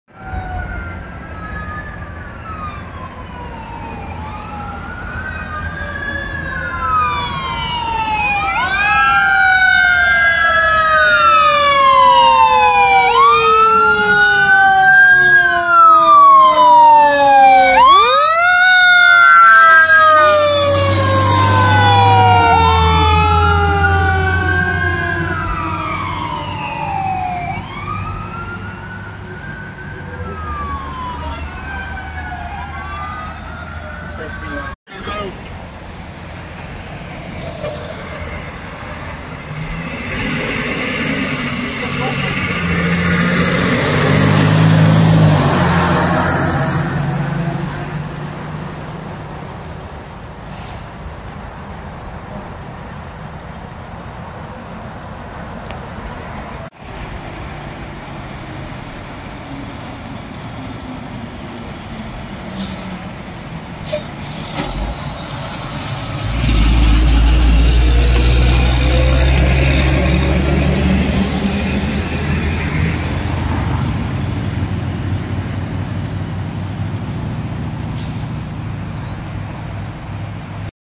Southeast Bullitt Fire Department Engine 6131, 6133 Tanker 6162 Responding
responding to a barn fire with mutual aid from Lebanon Junction.